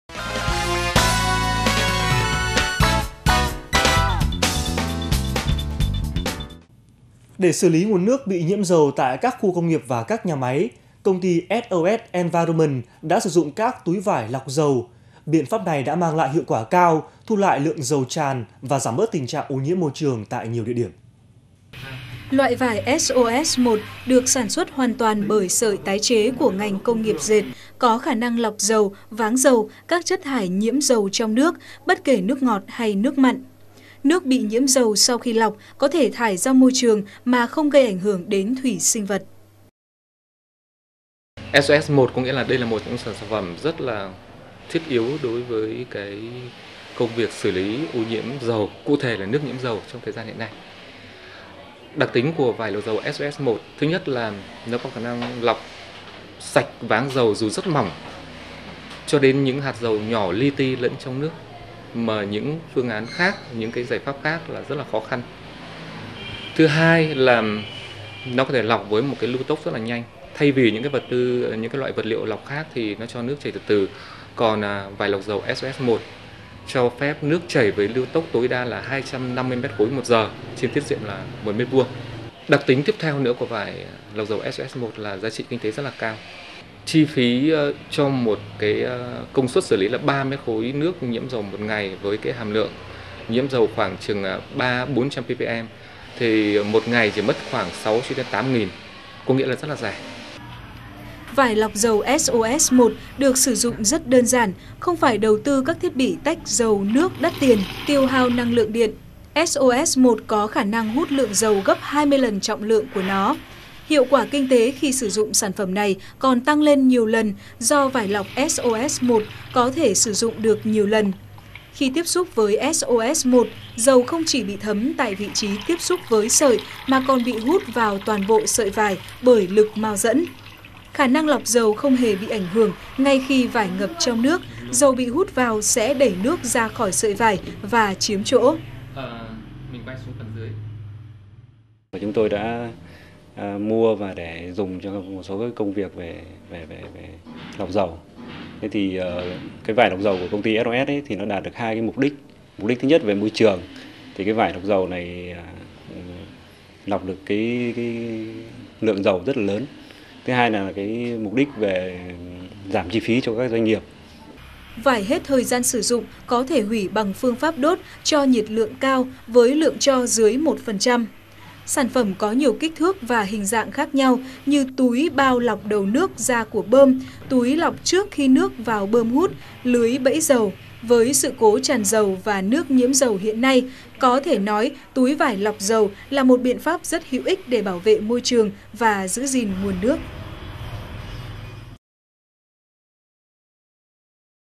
Vải lọc dầu SOS-1 trên kênh VTC10 - SOS Môi Trường
Vải lọc dầu SOS-1 được sản xuất từ 100% sợi tái chế của ngành công nghiệp dệt với đặc tính độc đáo: Sợi vải có khả năng lọc dầu, váng dầu, các chất thải nhiễm dầu trong nước (bất kể nước ngọt hay nước mặn). Vải chịu được dòng chảy với lưu tốc tối đa 250m3/giờ trên 1m2.

SOS-1 cung cấp giải pháp hiệu quả để giải quyết hàng loạt các vấn đề môi trường liên quan đến lọc và thu gom dầu, váng dầu, các chất thải nhiễm dầu trong nước có trong nước thải của các nhà máy, phân xưởng, cơ sở sản xuất, trạm sửa chữa cơ khí, cây xăng, nước nhiễm dầu ở cầu cảng, vịnh, nước đáy tàu nhiễm dầu...
Khi tiếp xúc với SOS-1, dầu không chỉ bị thấm tại vị trí tiếp xúc với sợi mà bị hút vào toàn bộ sợi vải bởi lực mao dẫn. Khả năng lọc dầu không hề bị ảnh hưởng ngay khi vải ngập trong nước, dầu bị hút vào sẽ đẩy nước ra khỏi sợi vải và chiếm chỗ.

SOS-1 có khả năng hút lượng dầu gấp 20 lần trọng lượng bản thân. Dầu bị hút vào sợi có thể dễ dàng tách ra bằng biện pháp cơ học (vắt, ép, tách ly tâm...), sau đó có thể sử dụng lại với hiệu quả giảm dần. Vải hết thời gian sử dụng có thể hủy bằng phương pháp đốt cho nhiệt lượng cao với lượng tro dưới 1%.
Sản phẩm có nhiều kích thước và hình dạng khác nhau: túi/bao lọc đầu nước ra của bơm, túi lọc trước khi nước vào bơm hút, lưới bẫy dầu, gối/tấm hút dầu cho miệng cống thoát nước/rãnh thoát nước, bạt phủ bờ hút dầu đồng thời tránh xói lở đất, rèm thu gom váng dầu tại bờ cảng...

Công ty TNHH SOS Môi Trường
Di Động: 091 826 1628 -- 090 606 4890
Showroom: P103 A5, Làng quốc tế Thăng Long, Trần Đăng Ninh, Dịch Vọng, Cầu Giấy, Hà Nội